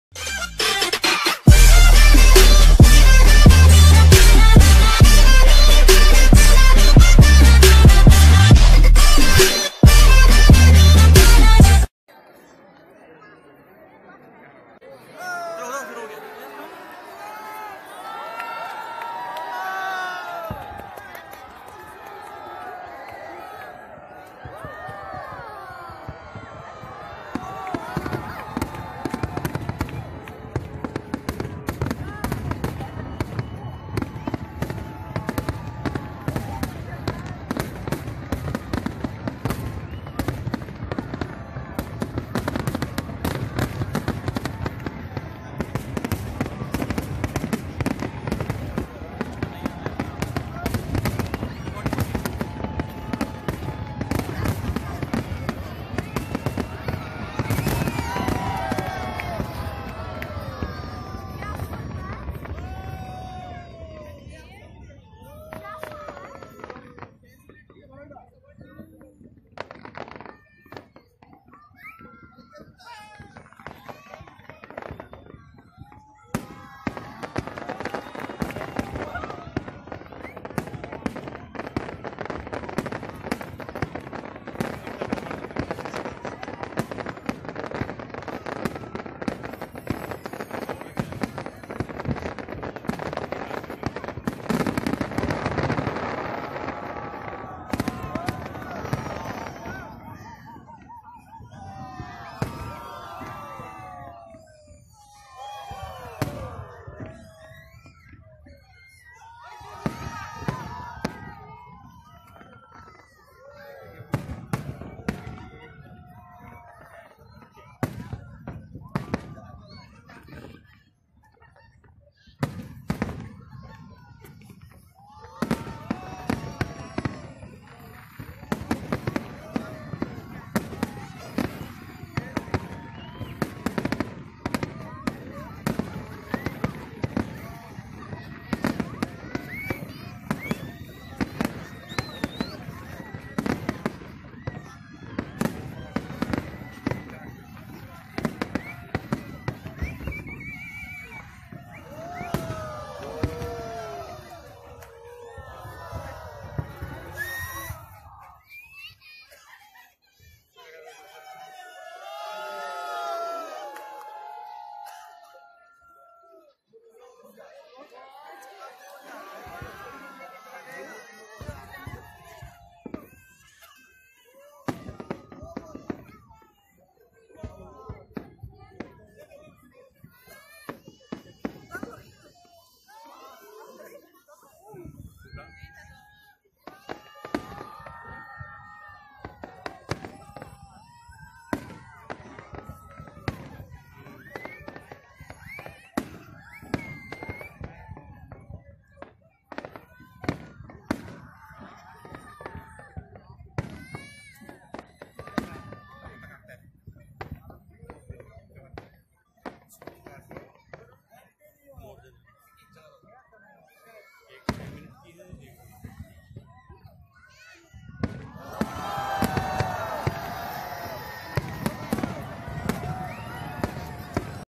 Wonderful Abu Dhabi Fireworks at New year 2018
Great Fireworks in Abu Dhabi at new year 2018